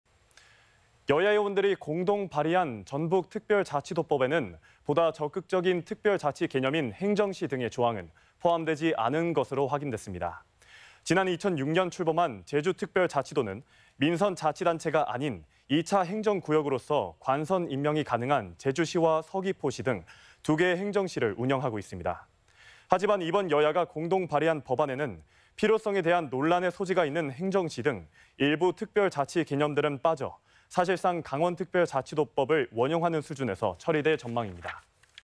전북특별자치도법 발의안에 행정시 등은 빠져 | 전주MBC 220825 방송
정태후기자의 더 많은 기사를 보고싶다면?

여야 의원들이 공동 발의한 전북특별자치도법에는 보다 적극적인 특별자치 개념인 행정시 등의 조항은 포함되지 않은 것으로 확인됐습니다.

지난 2006년 출범한 제주특별자치도는 민선 자치단체가 아닌, 2차 행정구역으로서 관선 임명이 가능한 제주시와 서귀포시 등 2개의 행정시를 운영하고 있습니다.

하지만 이번 여야가 공동발의한 법안에는 필요성에 대한 논란의 소지가 있는 행정시 등 일부 특별자치 개념들은 빠져 사실상 강원특별자치도법을 원용하는 수준에서 처리될 전망입니다.